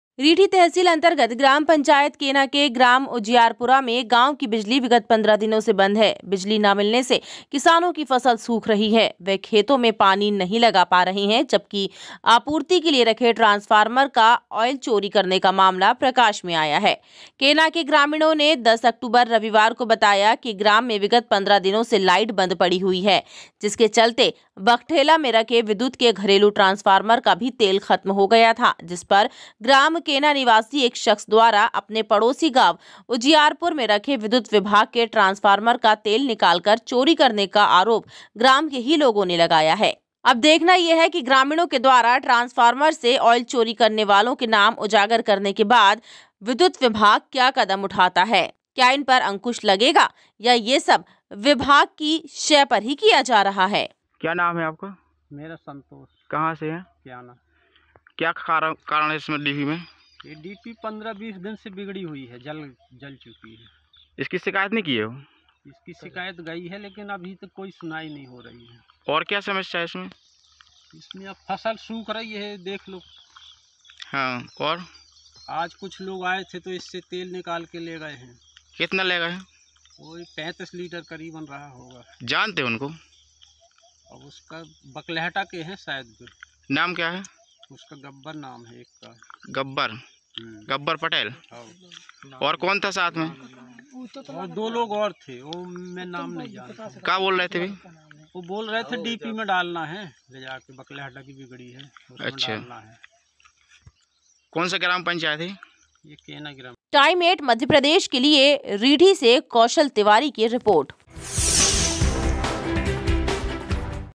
बिजली गुल किसान परेशान।TIME8 MADHYA PRADESH
कटनी। रीठी तहसील अंतर्गत ग्राम पंचायत केना के ग्राम उजियार पूरा मे गाव की बिजली विगत 15 दिनों से बंद है बिजली न मिलने से किसानों की फसल सूख रही है वे खेतो में पानी नही लगा पा रहे है।